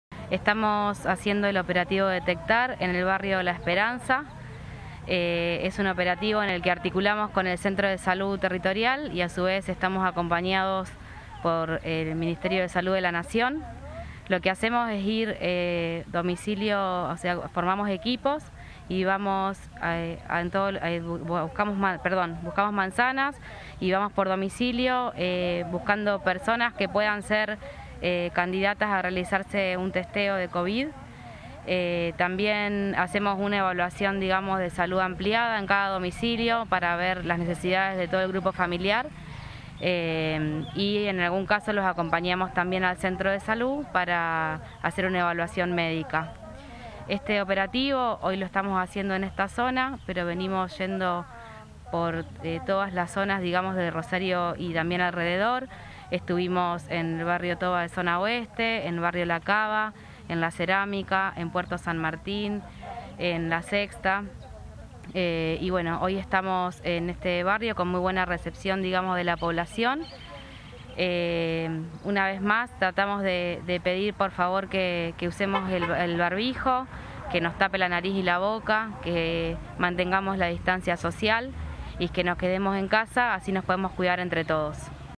Plan Detectar en más barrios de Rosario.
TAMBIÉN SE REALIZÓ EN CARLOS PELLEGRINI
LAURA ORTUBE – DIRECTORA PROVINCIAL DE SALUD

Plan detectar en Rosario y también en Carlos Pellegrini, donde por una fiesta clandestina se generó un foco de contagio. La tarea pasar domicilio por domicilio en busca de pacientes que tengan sintomatología relacionadas a COVID-19, para hacer hisopado y aislamiento temprano de todo el círculo familiar, como así también constatar otras necesidades de salud que pueda presentar la persona.